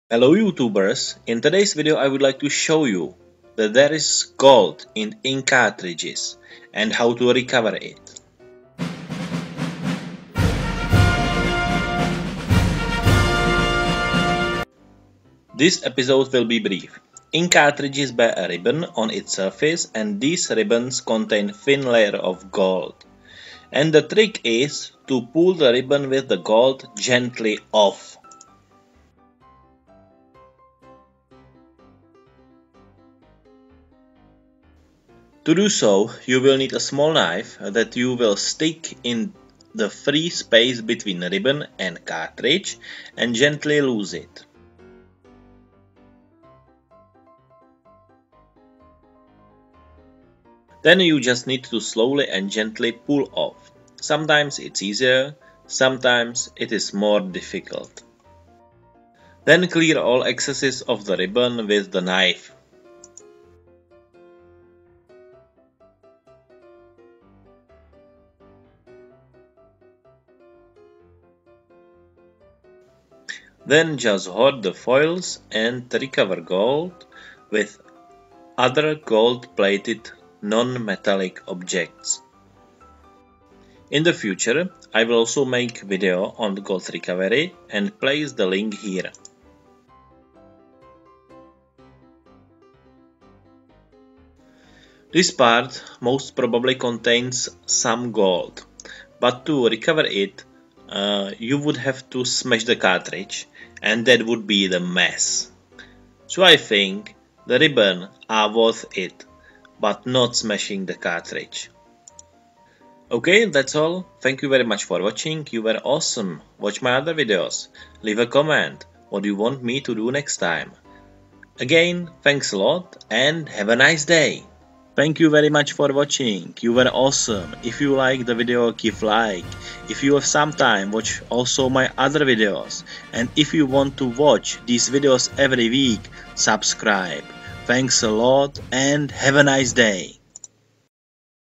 How to recover GOLD from recycled electronics: ink cartridges from printers
This video shows how to recover GOLD from ink cartridges from printers